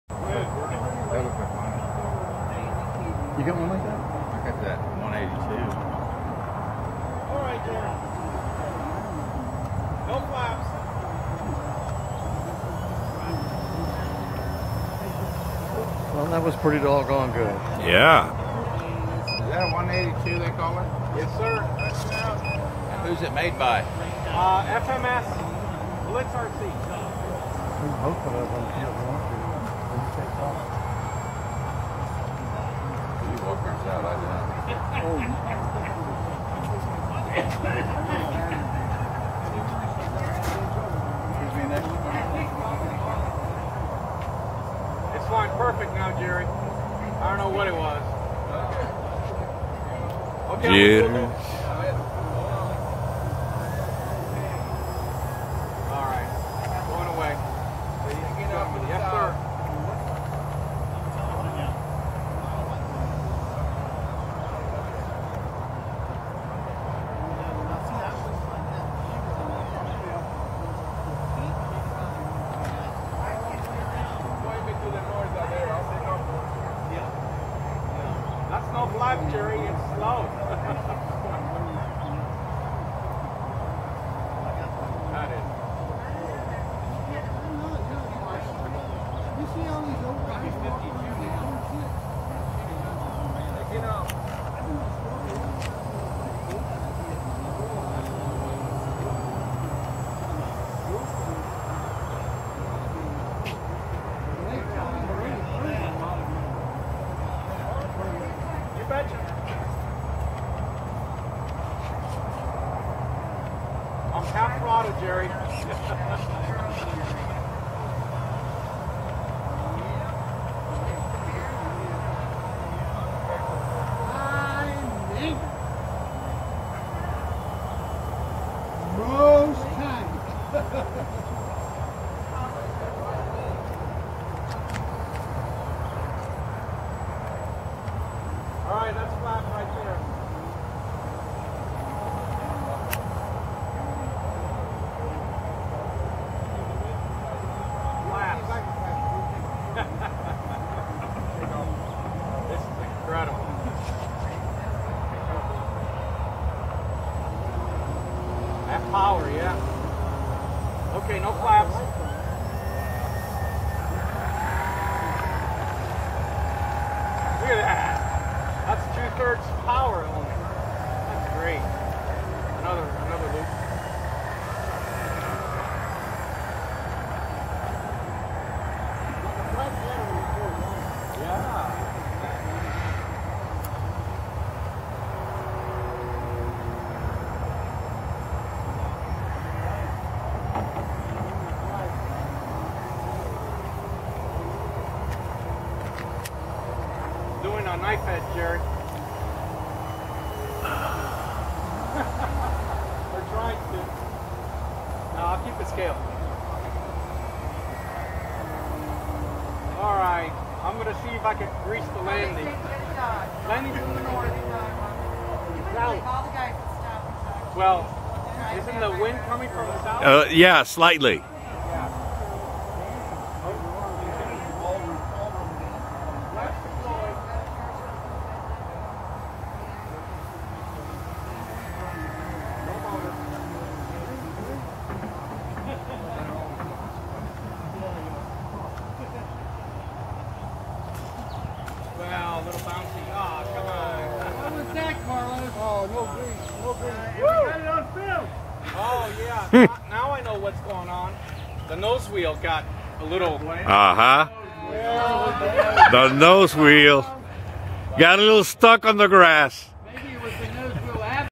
BlitzRC - FMS Cessna 182 4th Flight over Moon Port Modelers RC Club 08-08-2012
4th Flight of my new Cessna 182 55-Inch Wing Span over Moon Port.  Rather hard landing due to Dead Stick with Flaps!  Need to keep slight power with flaps on landing and especially with dead calm conditions.